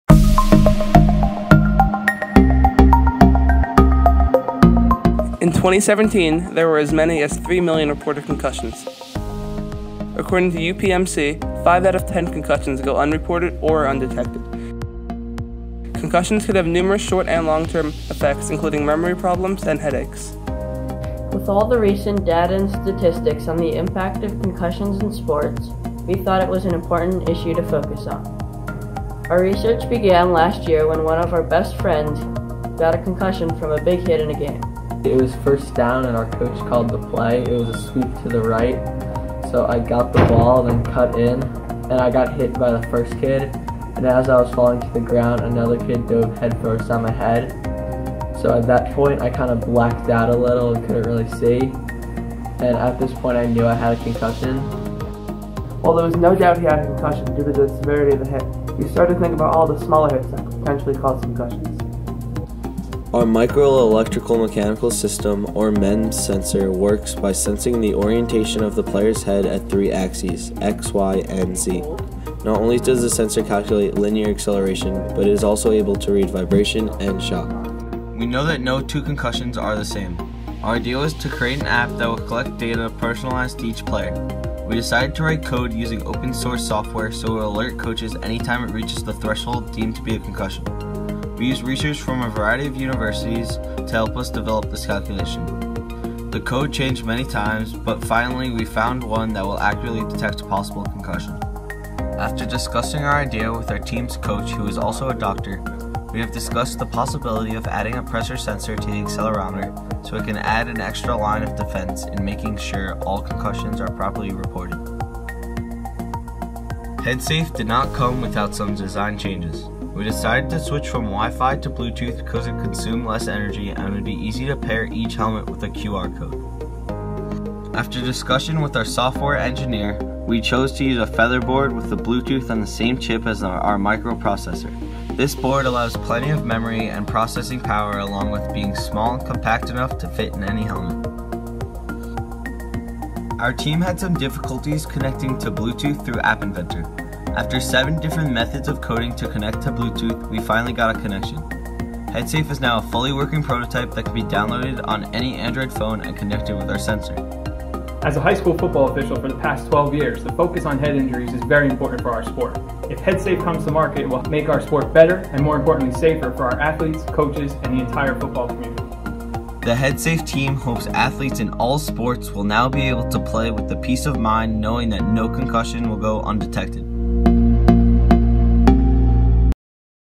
Cavallini Middle School, New Jersey
Reported concussions reached upwards of 3 million in 2017, and it’s estimated that half of all concussions went unreported or undetected. After a classmate suffered a concussion during football season last year, students at Cavallini Middle school developed sensors to be worn on football helmets to accurately and quickly detect concussions and help prevent any further brain damage through early detection.